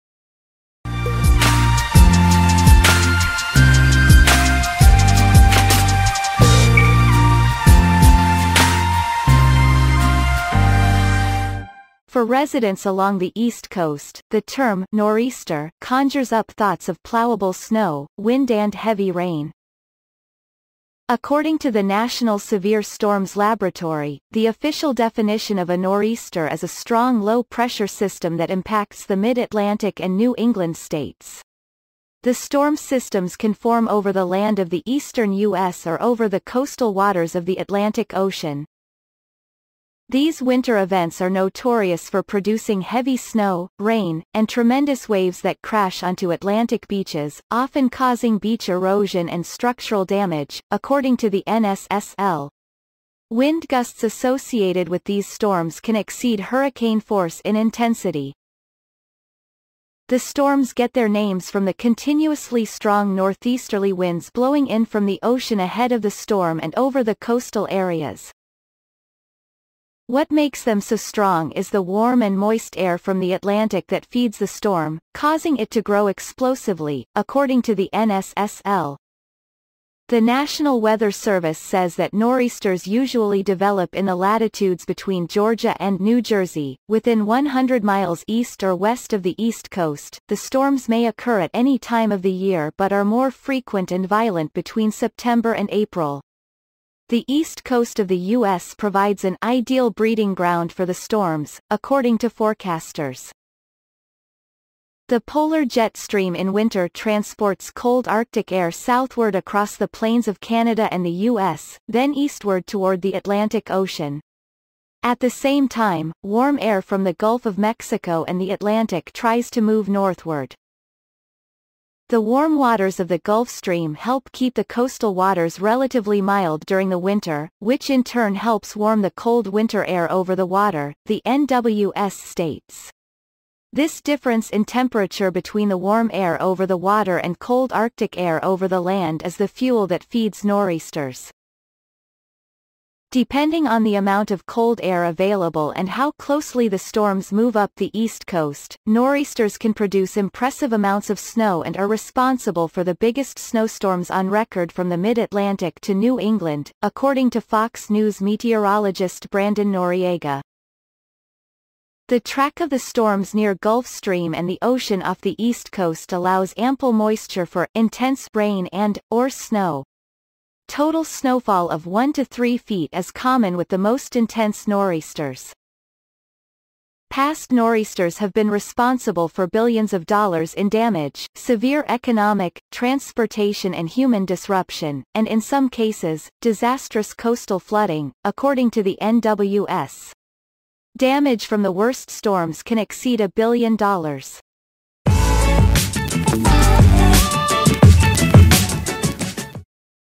What is a nor'easter? The classic East Coast storms explained
For residents along the East Coast, the term "nor'easter" conjures up thoughts of plowable snow, windand heavy rain.  According to theNational Severe Storms Laboratory, the official definition of a nor'easter is a strong low-pressure system that impacts the Mid-Atlantic and New England states. The storm systems can form over the land of the eastern U.S. or over the coastal waters of the Atlantic Ocean.  "These winter events are notorious for producing heavy snow, rain, and tremendous waves that crash onto Atlantic beaches,...